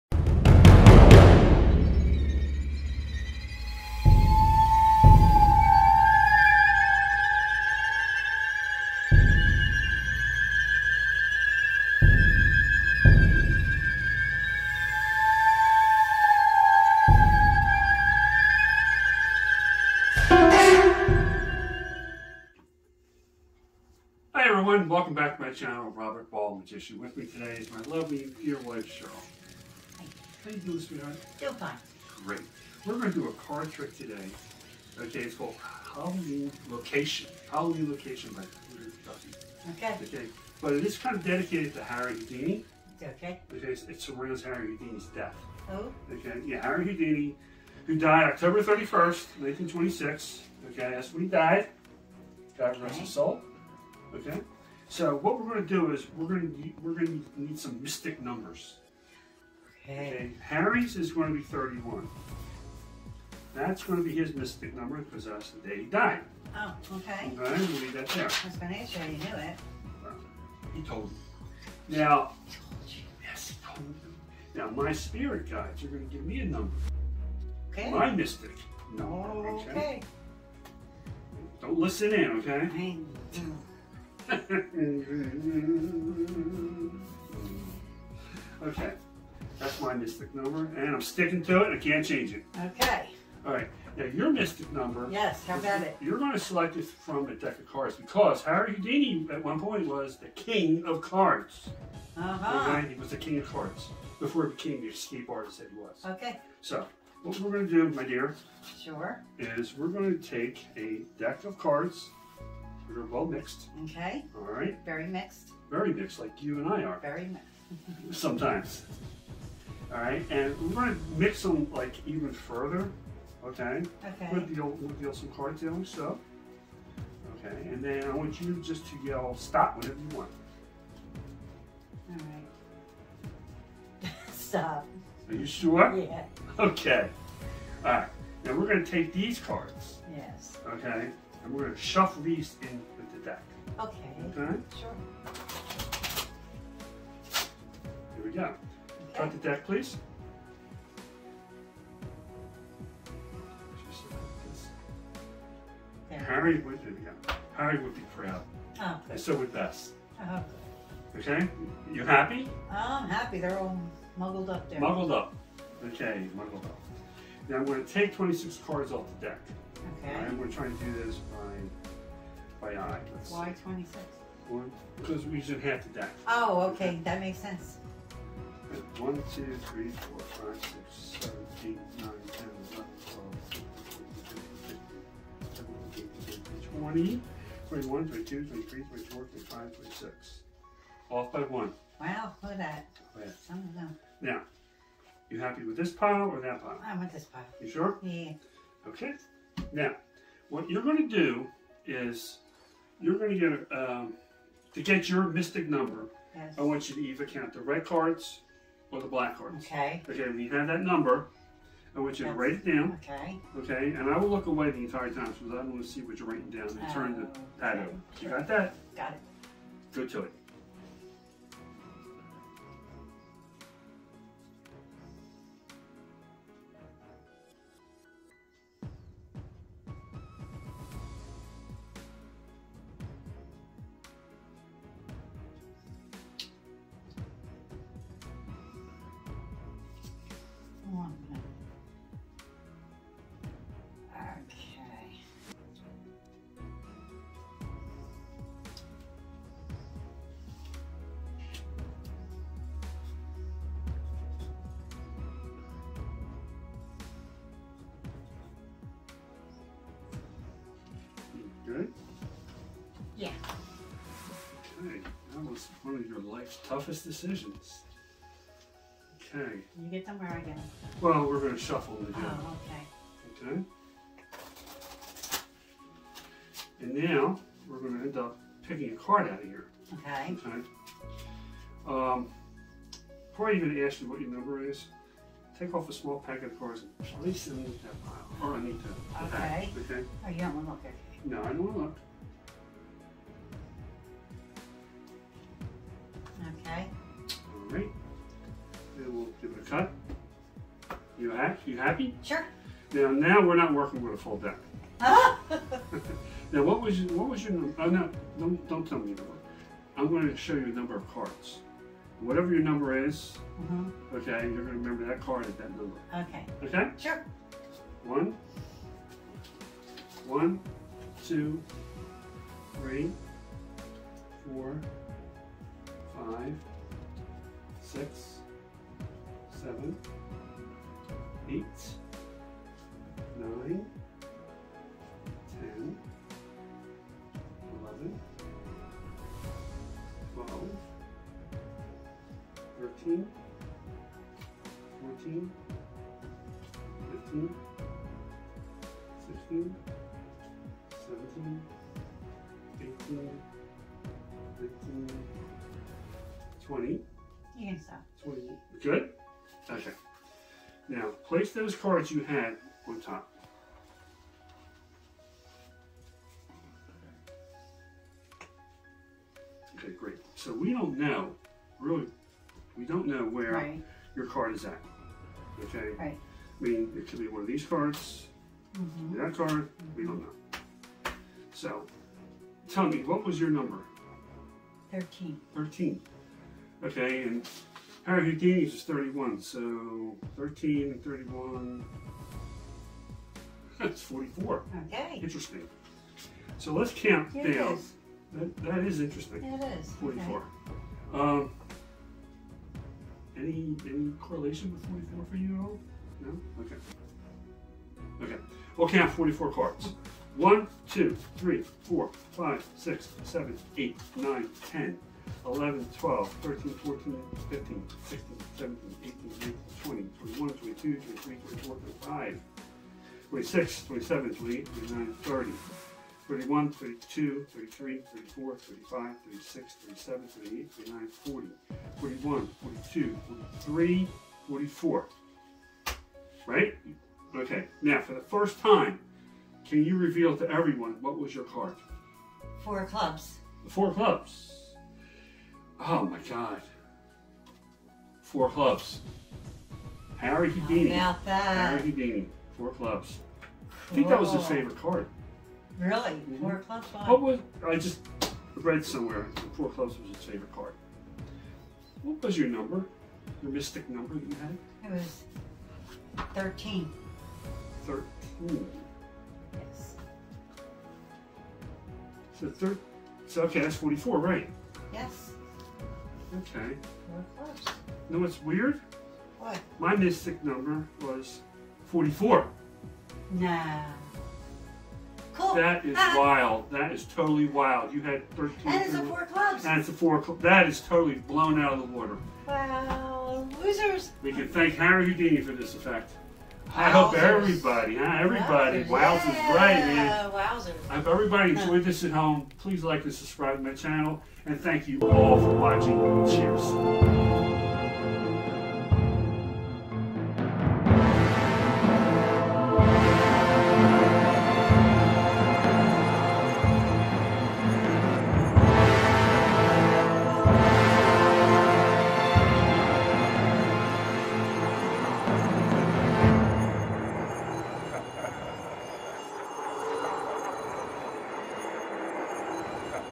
Halloween Location inspired by Harry Houdini and Peter Duffie Performance 👻😱 💀👽👾🙀👀🎩😈🪄
It’s that time of year again HALLOWEEN. Sit back and enjoy the Spirit of Spooky. 👻😱 💀👽👾🙀👀🎩😈🪄